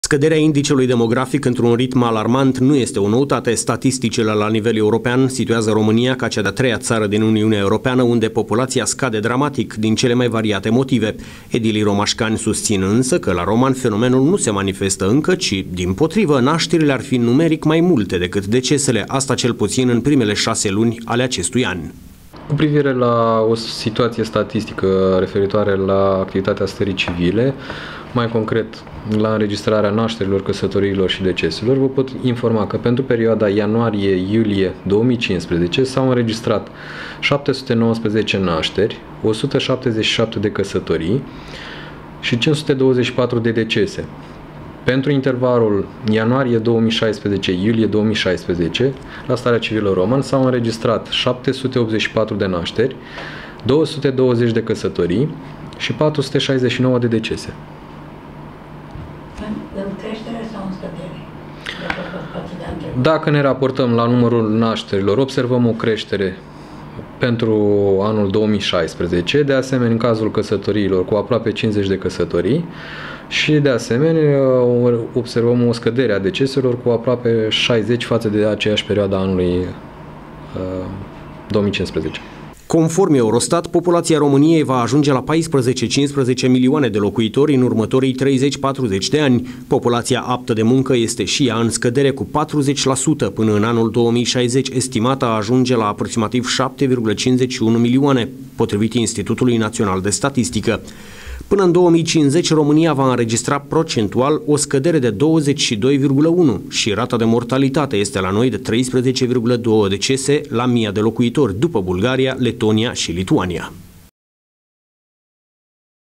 NE ÎMPUȚINĂM CONSIDERABIL
Roman24 peste tot! Scăderea indicelui demografic într-un ritm alarmant nu este o noutate. Statisticile la nivel european situează România ca cea de a treia țară din Uniunea Europeană unde populația scade dramatic, din cele mai variate motive. Edilii romașcani susțin însă ca la Roman fenomenul nu se manifestă încă, ci...dimpotrivă, nașterile ar fi numeric mai multe decît decesele, asta cel puțin în primela șase luni ale acestui an.